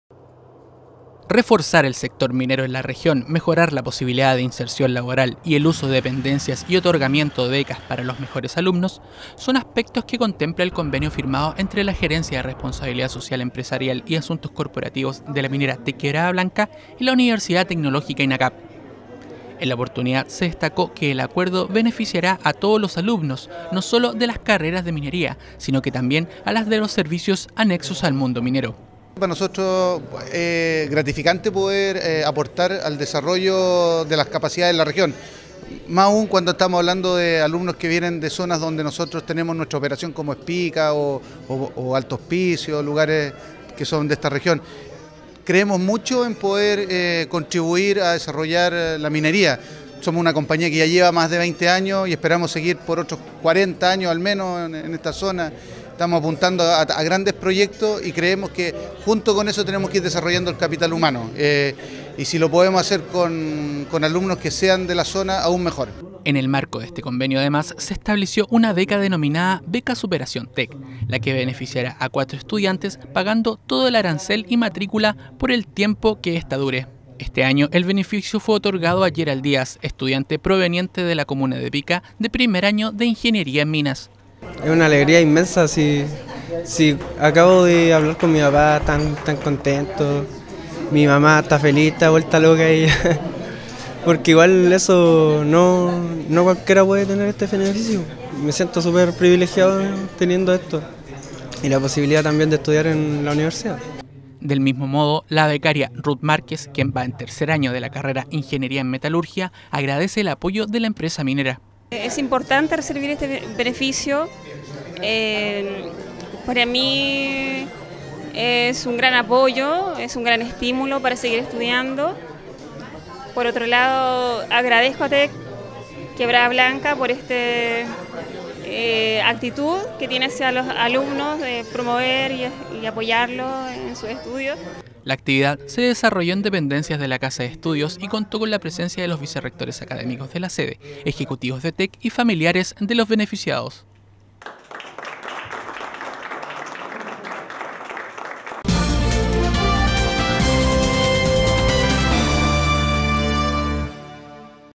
EMPRESA MINERA TECK QB FIRMA CONVENIO DE COLABORACIÓN CON INACAP - Iquique TV
(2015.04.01) En el marco del trabajo realizado por la minera TECK QB, la empresa cuprífera firmó este miércoles un convenio de colaboración con INACAP Iquique, que beneficiará a los alumnos de las carreras ligadas al mundo productivo minero.